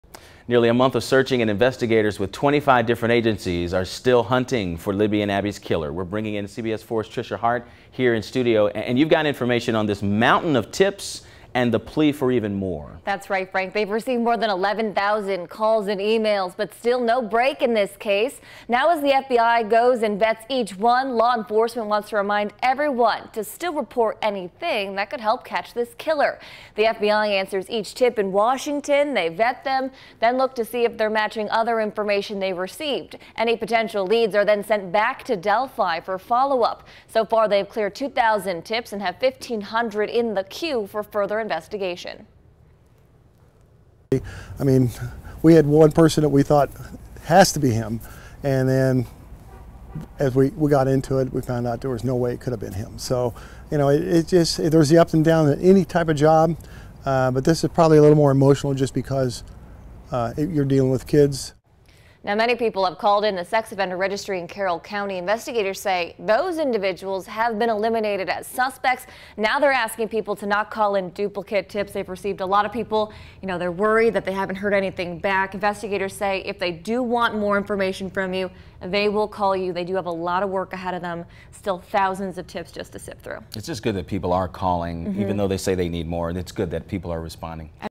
CBS4 Live Report: Police urge tips in double murder investigation, nearly 1 month later
Two teens were found murdered in the woods of Carroll County. Investigators with local law enforcement and the FBI are sorting through 11,000+ tips, but still ask for any and all information that could help solve the case.